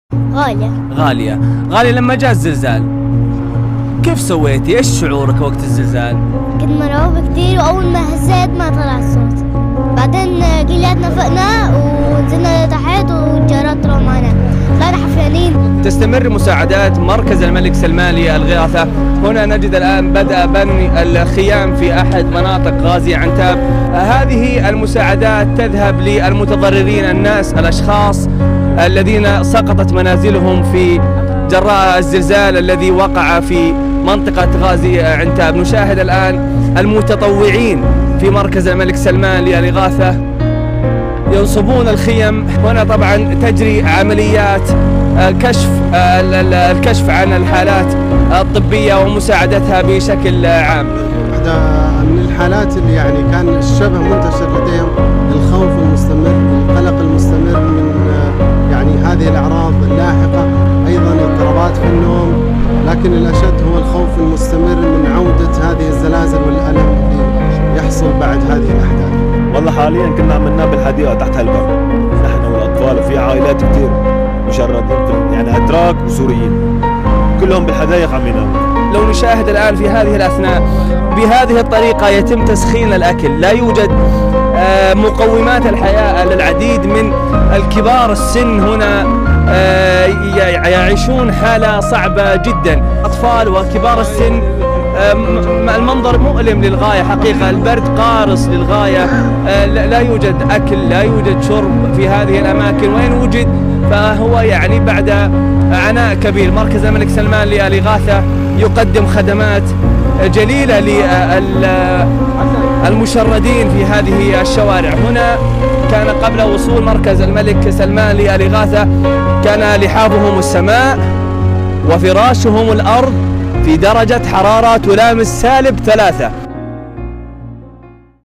طفلة تروي لحظاتها الأولى في الزلزال
الطفلة غالية وآخرون يروون اللحظات الأولى أثناء وقوع الهزة لموفد ⁧‫الإخبارية‬⁩ عبدالله الرويس